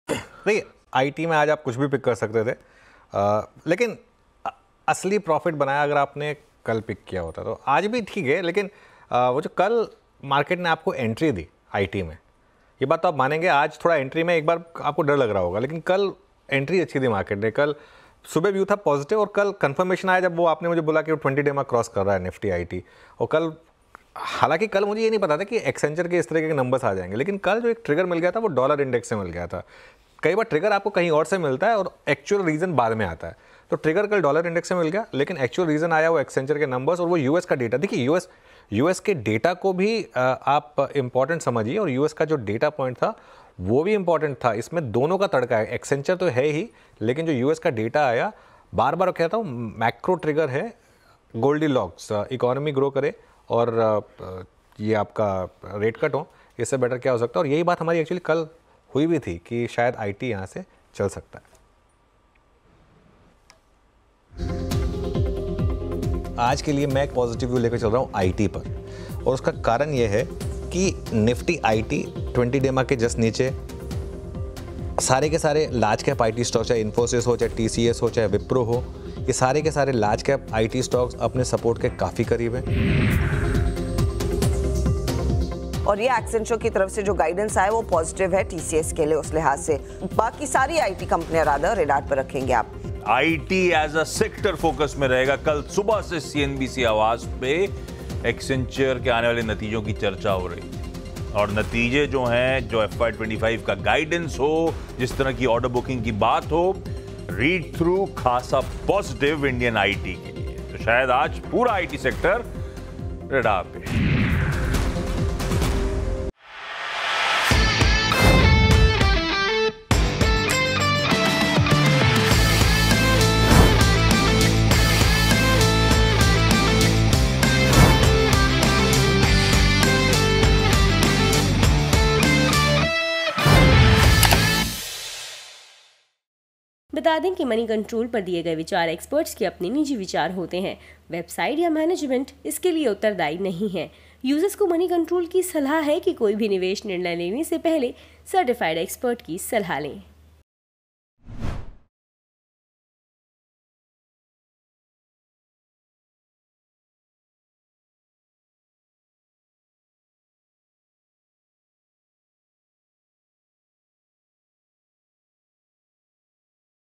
IT Sector | IT Stocks Picks | IT Stocks में किसे चुनना है बेहतर!
IT सेक्टर के किन शेयरों में करनी चाहिए खरीदारी. जानें किन IT stocks को चुनना है बहतर. इन शेयरों में कल खरीदारी कर चुके निवेशकों ने क्या आज मुनाफा बनाया. जानने के लिए देखें ये वीडियो.